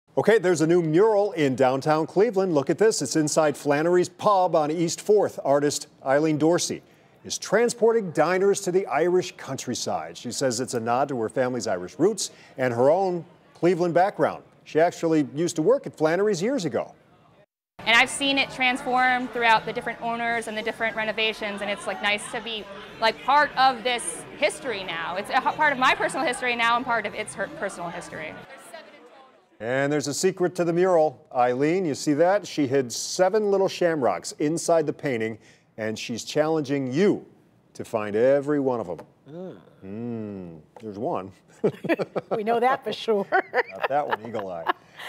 Award-winning artist returns to her roots to create mural at Flannery's Pub
An award-winning artist and former server at the historic Flannery’s Pub on East 4th Street returned to her former employer to create a large mural inside the restaurant and bar.